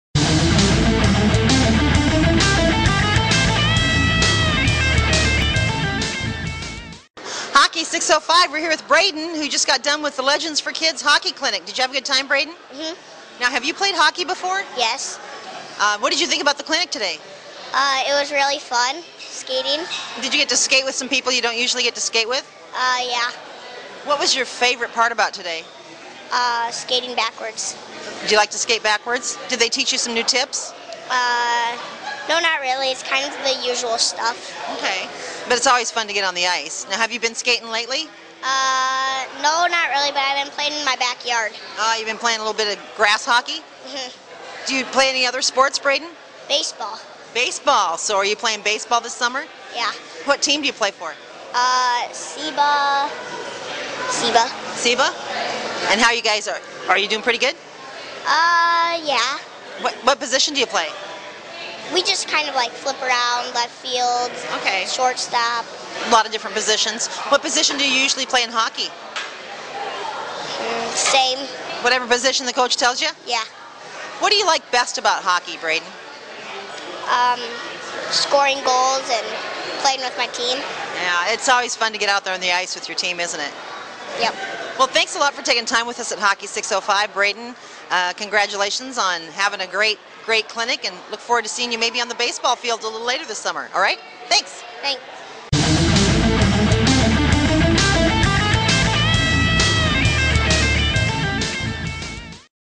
Braydon at LegendsForKids Hockey Clinic
June 13, 2015 in Sioux Falls, SD - Hockey605 stopped by the Sanford/HyVee Legends For Kids Hockey Clinic at Scheels IcePlex, and got a chance to interview a few of the participants. Here is our interview with Braydon.